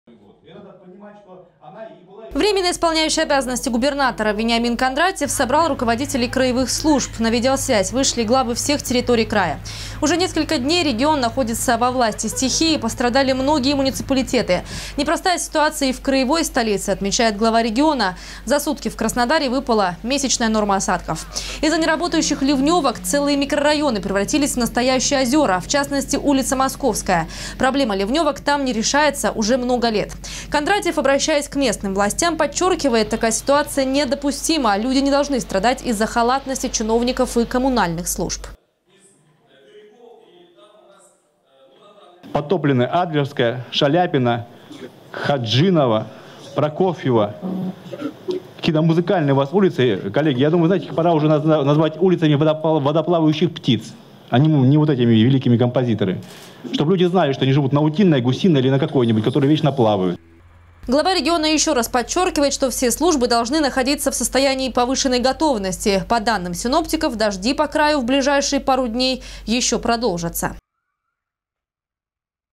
Глава региона призвал активизировать работу всех служб из-за непогоды. Новости Сочи Эфкате
В администрации сегодня прошло селекторное совещание комиссии по чрезвычайным ситуациям. Временно исполняющий обязанности губернатора Вениамин Кондратьев собрал руководителей краевых служб.
Сочи. Новости.
Телеканал Эфкате.